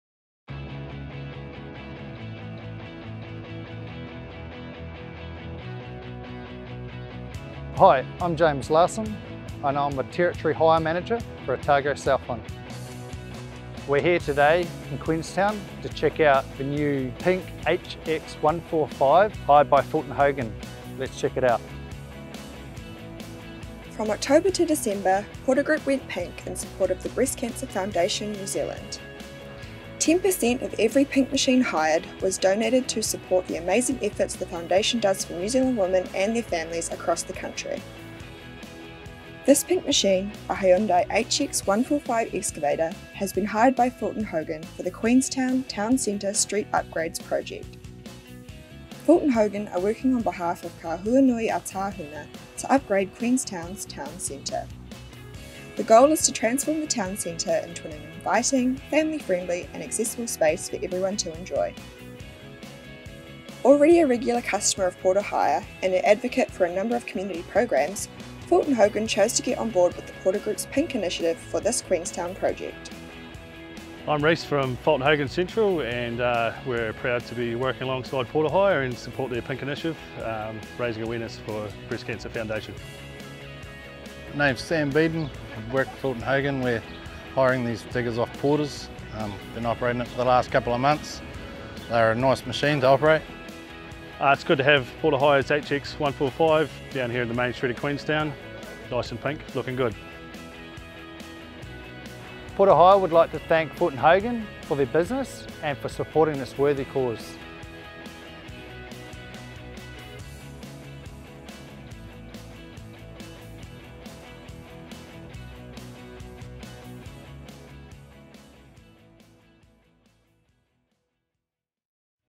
Customer Spotlight I Fulton Hogan Goes Pink
This week, Porter Hire headed to beautiful Queenstown to catch up with @Fulton Hogan and take a look at one of our portergroupgoespink machines working in the town centre.
This HX145 excavator hire is operating as part of our pink initiative raising money for the @Breast Cancer Foundation New Zealand.

A big thank you to the Fulton Hogan team for having us on site and for supporting this cause.